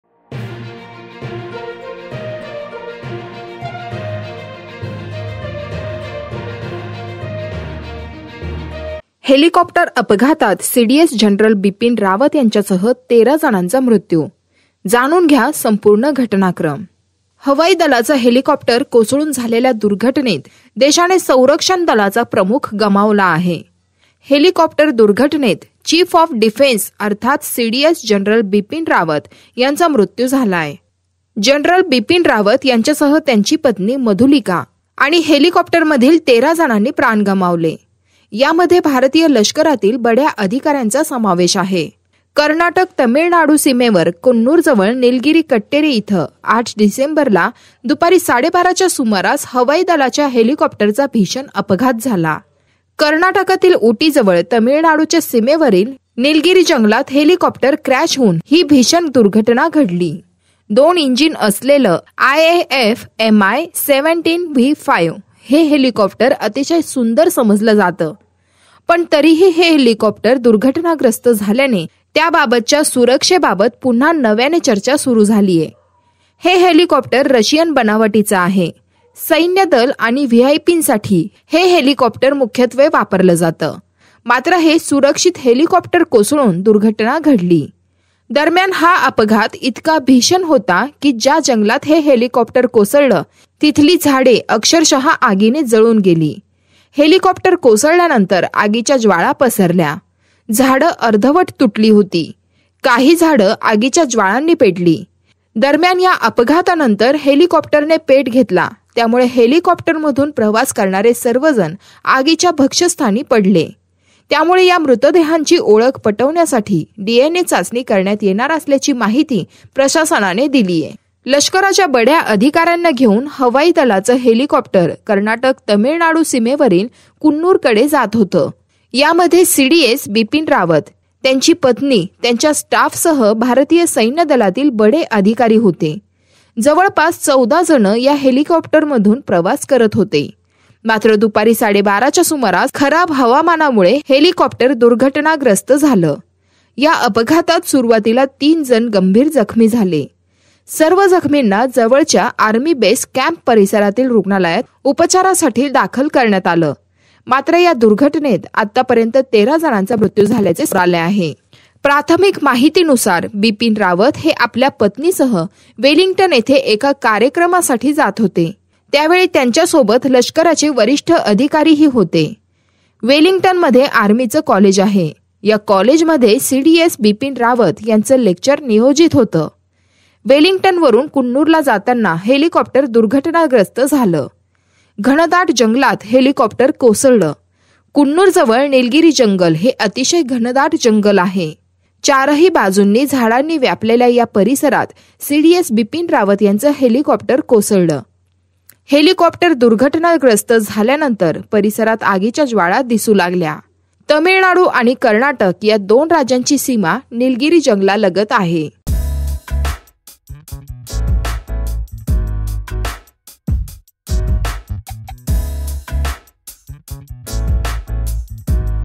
CDS Bipin Rawat helicopter crash: All about IAF's Mi-17V5 chopper
Wing Commander Prithvi Singh Chauhan was pilot of the Mi-17V5 which crashed with 14 personnel on board including CDS Gen Bipin Rawat. He is the Commanding Officer of the 109 Helicopter Unit.

Gen Rawat, his wife and 11 others killed in IAF chopper crash